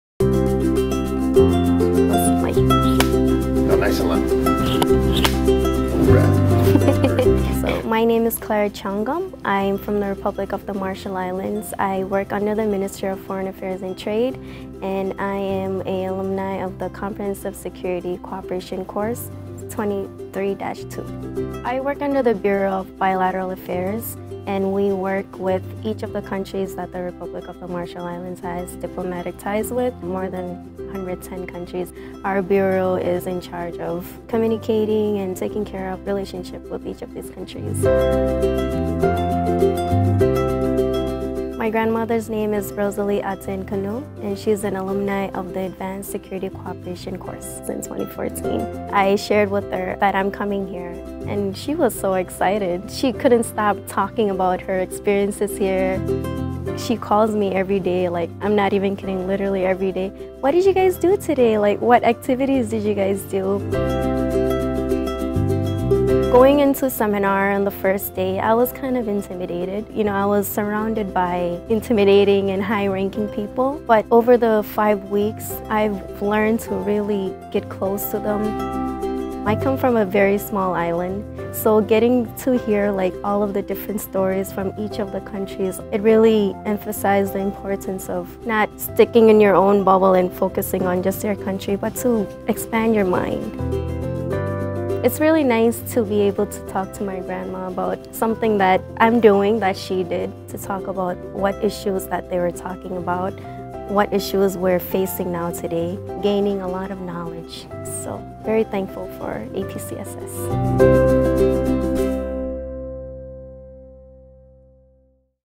Fellow's Spotlight: Claret Chungum from the Marshall Islands
With more than 25 years of educating, empowering, and connecting security practitioners around the world, we’re now seeing participants whose parents or even grandparents attended a course at the Daniel K. Inouye Asia-Pacific Center for Security Studies.

Claret Chongum recently graduated from the Comprehensive Security Cooperation (CSC) course 23-2 on June 29. Hailing from the Marshall Islands, she works as a Foreign Service Officer in the Ministry of Foreign Affairs and Trade. Her grandmother, Rosalie Konou, is also an alumna of the Center, having graduated from the Advanced Security Cooperation course in 2014.

Claret’s grandmother attended the course when Claret was in high school. When she was invited to participate in a course at the same institution, she was thrilled to share the news with her retired grandmother, the first female lawyer in the Marshall Islands.

“She was so excited,” said Claret. “She couldn’t stop talking about her experiences and the friends she made from other countries.”

When Claret took the course, she and her grandmother spoke every night, discussing lectures, activities, and critical issues affecting their homeland, such as climate change and illegal, unregulated, and unreported fishing. Claret shared what she was learning from Fellows from various countries like Palau, Pakistan, Indonesia, and Mongolia. Her grandmother helped to focus her Fellows’ Project. Her project aims to enhance the exit strategy for Marshall Islands nationals living in Taiwan in the event of an armed conflict.

Claret’s grandmother remained in touch with her friends from the Center even after almost a decade, and Claret plans to do the same. She feels grateful for the shared experience with her grandmother and values the friendships and knowledge gained during the course. For Claret, the connections made during this 5-week experience have enriched her life.

Said Claret, “[From this course] I feel fortunate to now have more friends, an even greater network, and even greater knowledge on very important and relevant issues.”

To learn more about DKI APCSS visit dkiapcss.edu.